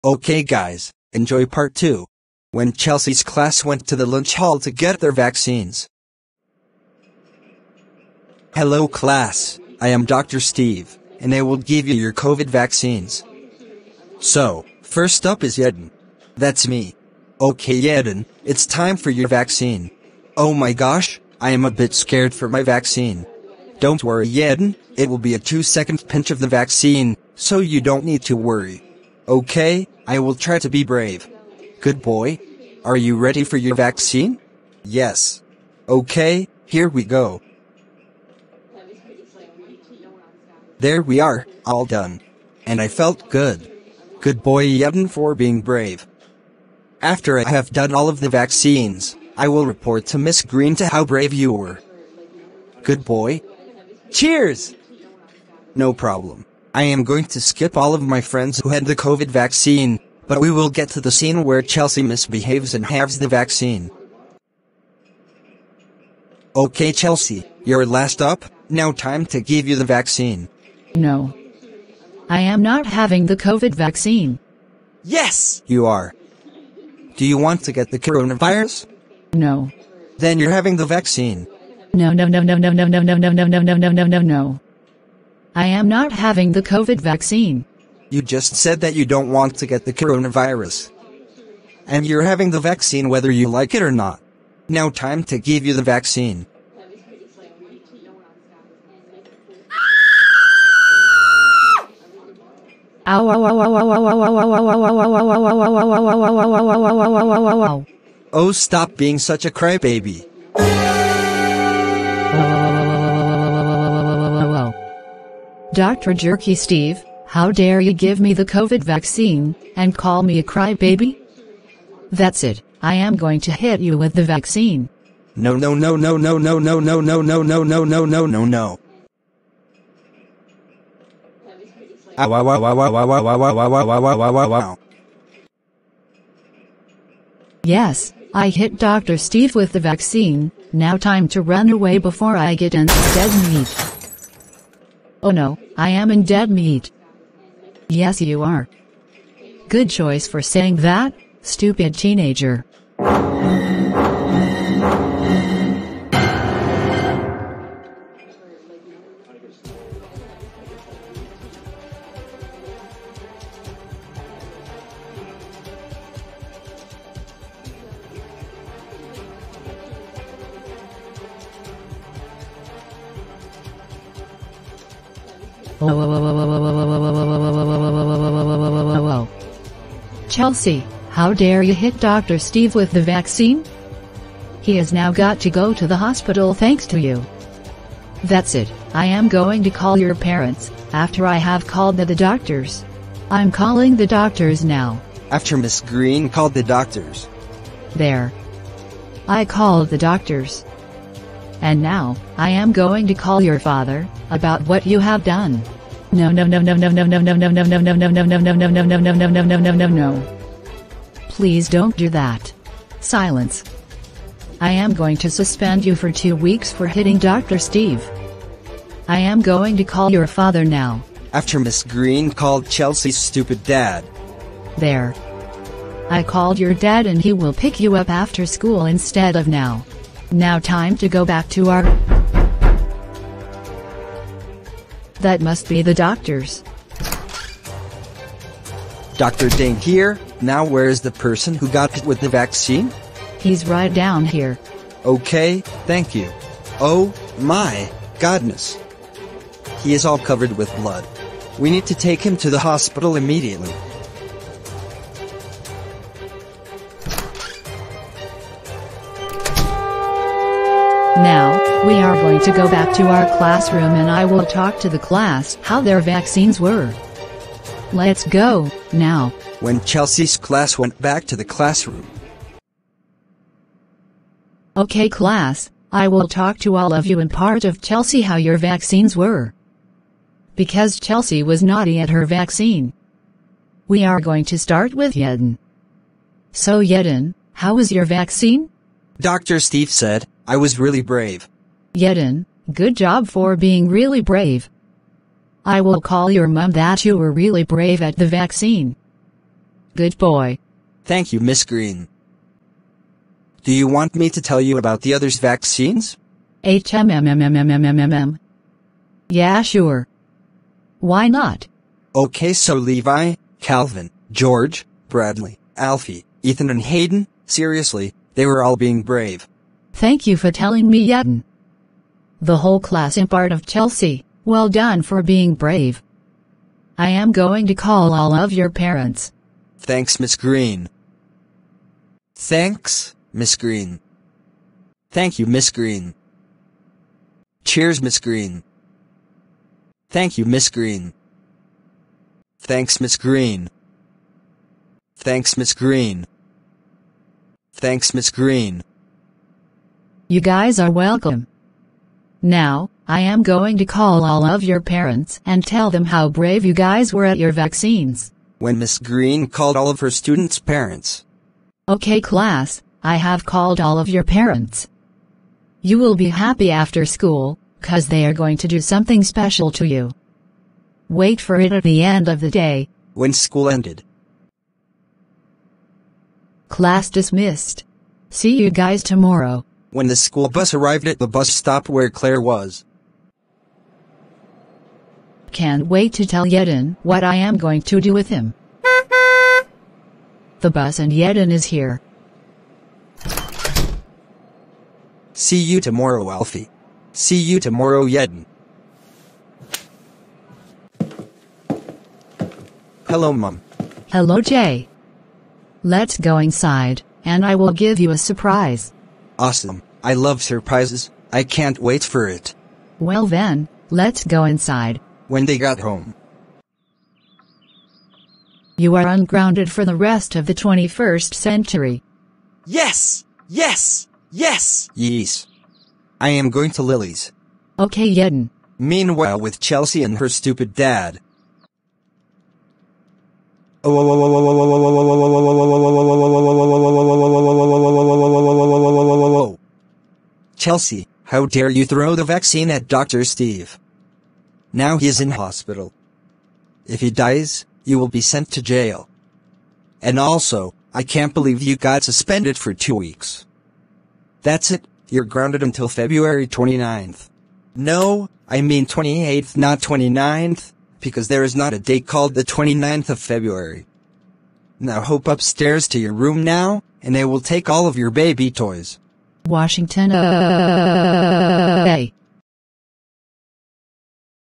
Jaeden Behaves at His Covid Vaccine in School/Ungrounded/Chelsea Misbehaves at The Vaccine/...
Grounded - Part2 - Series 1 Episode 8